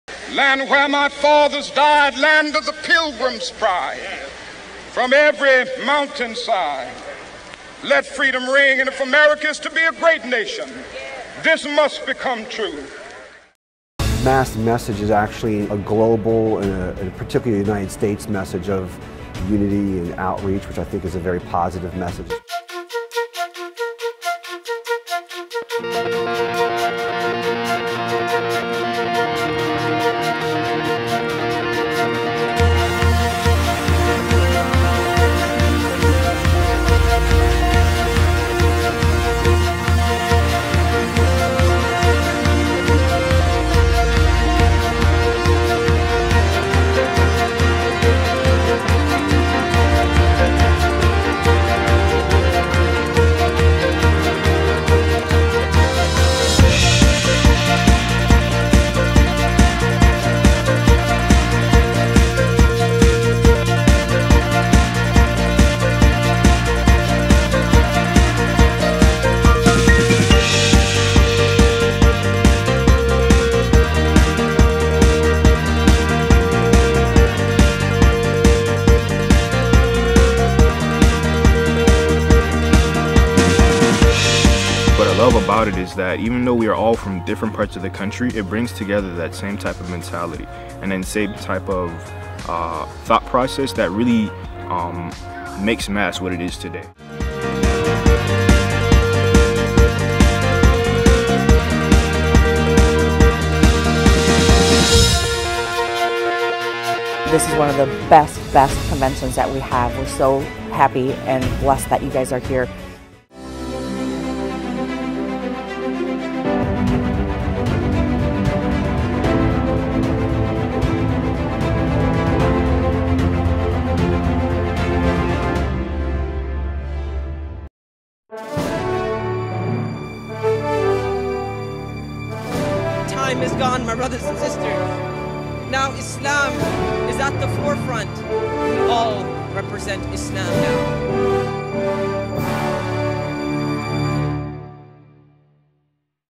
MAS - What We Do
This video shows a glimpse of what the Muslim American Society has to offer.